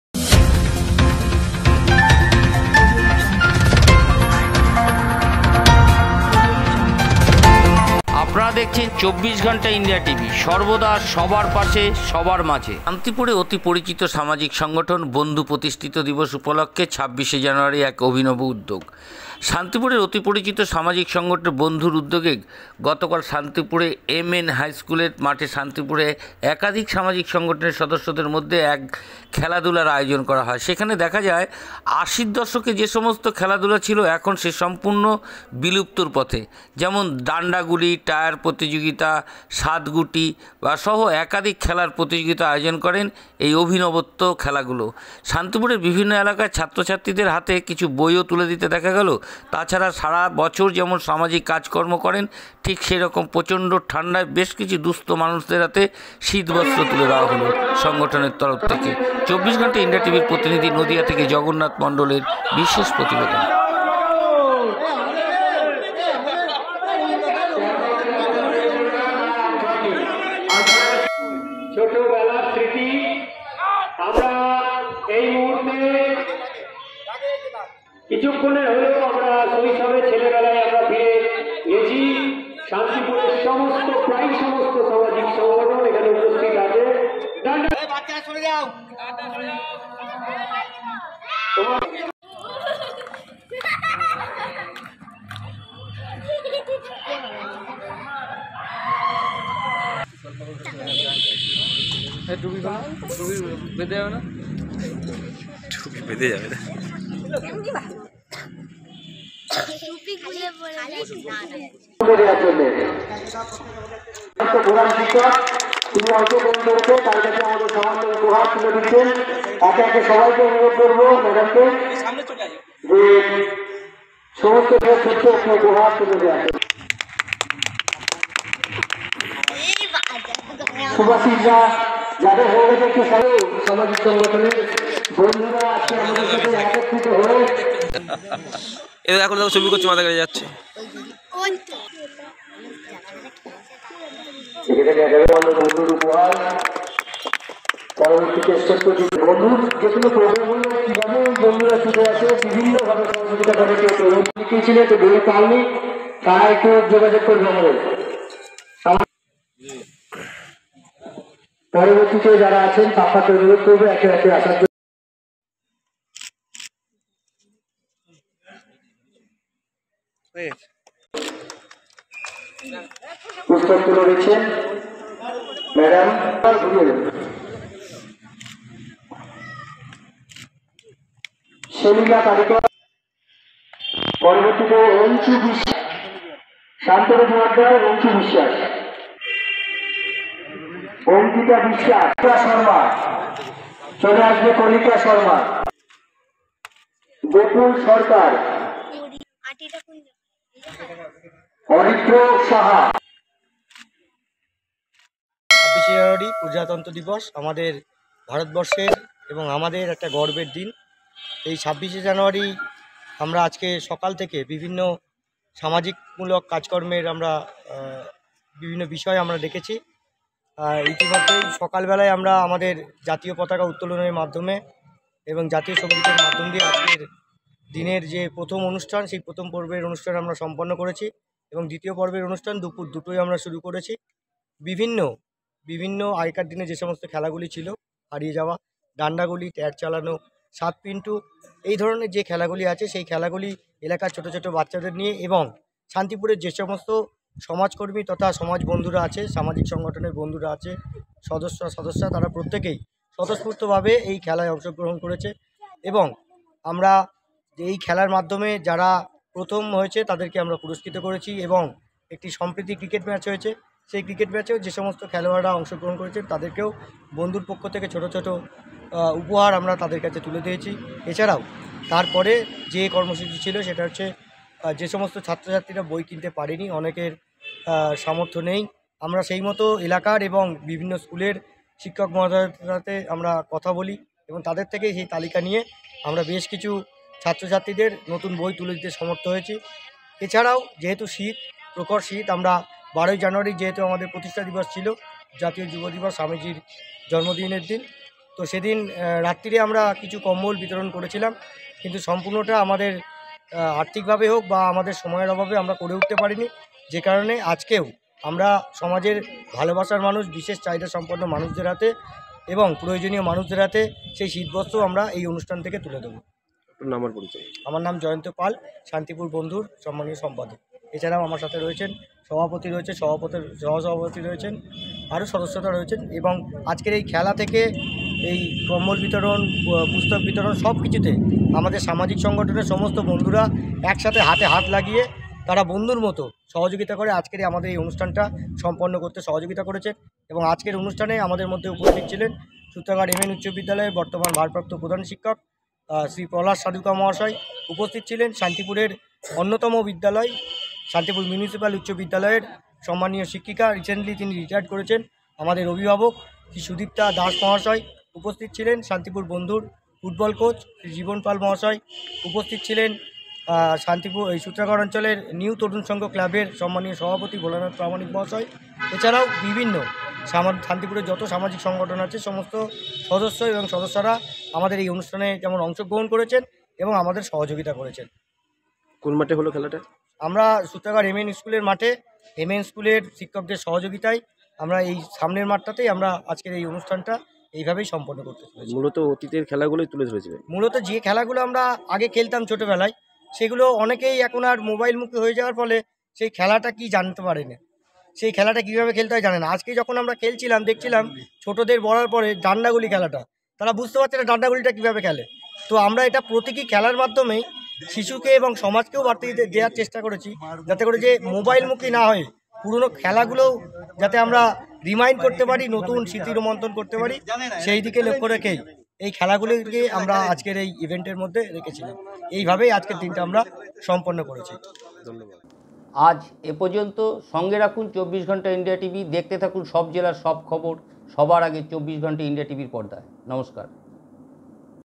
24ghantaindiatv শান্তিপুর অতি পরিচিত সামাজিক সংগঠন বন্ধু প্রতিষ্ঠা দিবস
24ghantaindiatv শান্তিপুর অতি পরিচিত সামাজিক সংগঠন বন্ধু প্রতিষ্ঠা দিবস উপলক্ষে 26 শে জানুয়ারি এক অভিনব উদ্যোগ
শান্তিপুরে অতি পরিচিত সামাজিক সংগঠন বন্ধুর উদ্যোগে গতকাল শান্তিপুর এমএন হাইস্কুলের সামনে শান্তিপুরের একাধিক সামাজিক সংগঠনের সদস্যদের মধ্যে এক খেলাধুলার আয়োজন করা হয় সেখানে দেখা যায় আশির দশকে যে সমস্ত খেলাধুলা ছিল তাই এখন সম্পূর্ণই বিলুপ্ত। যেমন দান্ডা গুলি টায়ার প্রতিযোগিতা সাত বাট্টু সহ একাধিক খেলার প্রতিযোগিতার আয়োজন করেন এবং শান্তিপুরের বিভিন্ন এলাকার ছাত্র-ছাত্রীদের হাতে কিছু বইও তুলে দিতে দেখা গেল তাছাড়াও সারা বছর যেমন সামাজিক কাজকর্ম করেন ঠিক সেই রকম প্রচন্ড ঠান্ডায় বেশ কিছু দুস্থ মানুষের হাতে শীতবস্তু তুলে দিলেন

অনুষ্ঠান শেষে সামাজিক সংগঠন বন্ধু পরিবারের সম্পাদক কি জানালেন শোনাবো আপনাদের